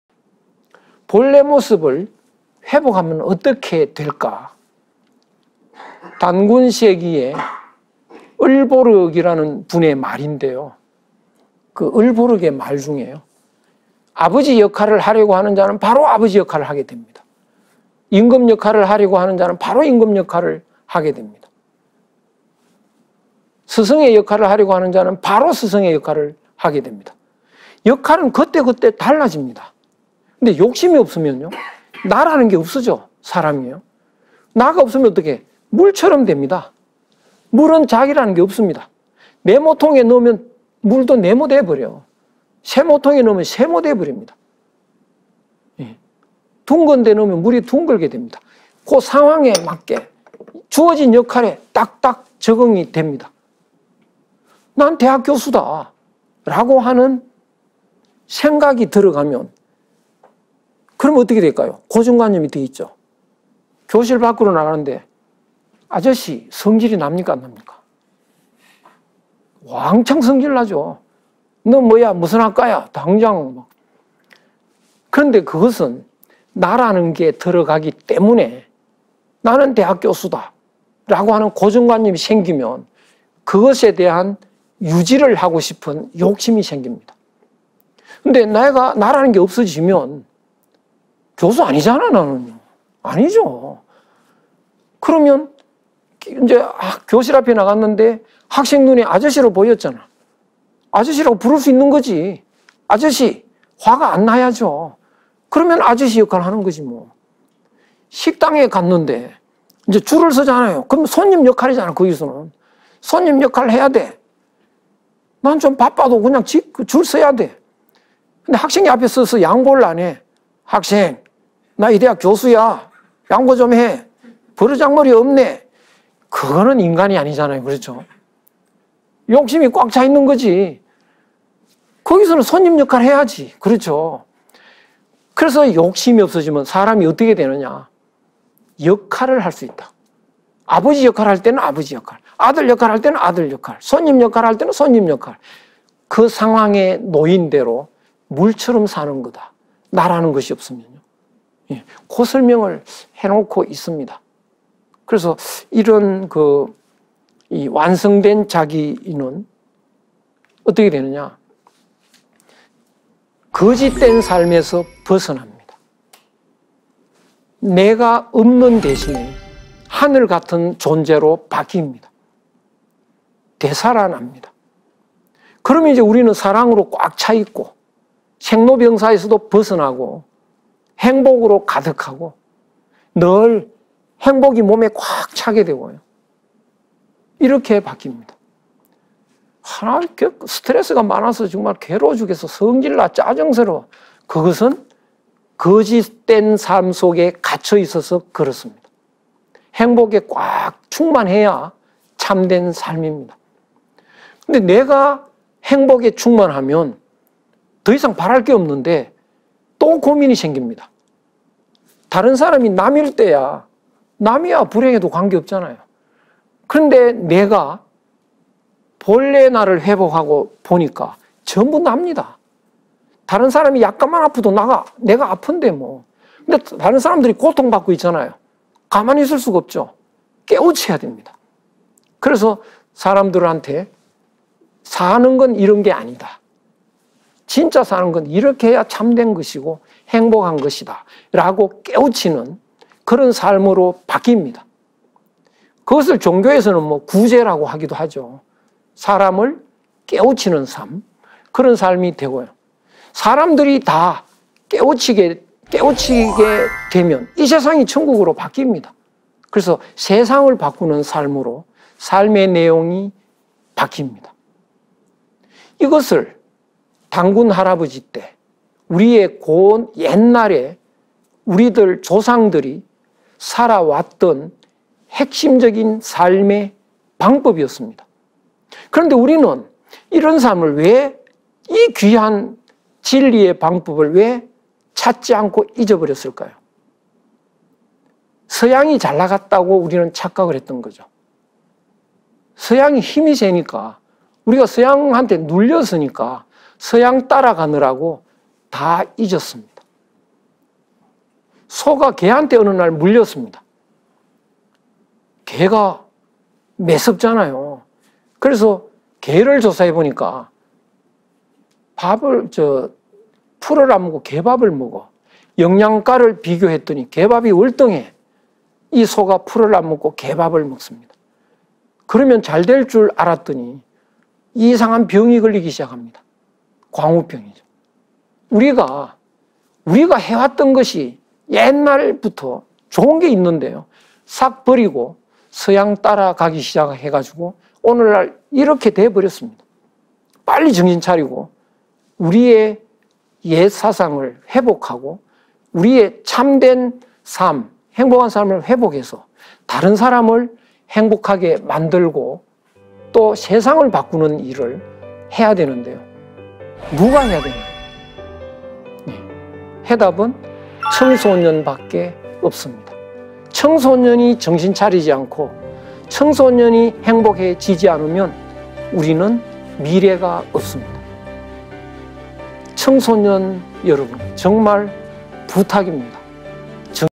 생로병사에서 벗어나는 법ㅣ완성된 사람이 사는 세상은 어떨까ㅣ단군세기 을보륵ㅣ이기동ㅣSTB상생방송
STB청소년특강 마음자리 34ㅣ한국 사상의 기원 2부 성균관대 유학동양학 이기동 교수